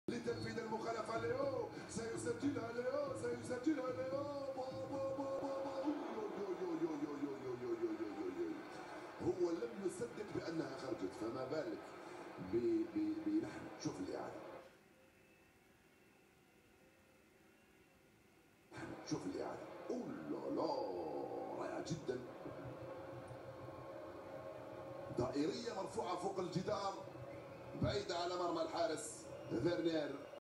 Barcelona VS Hueska. 76th minute. Messi’s free kick
Almost scored. Messi again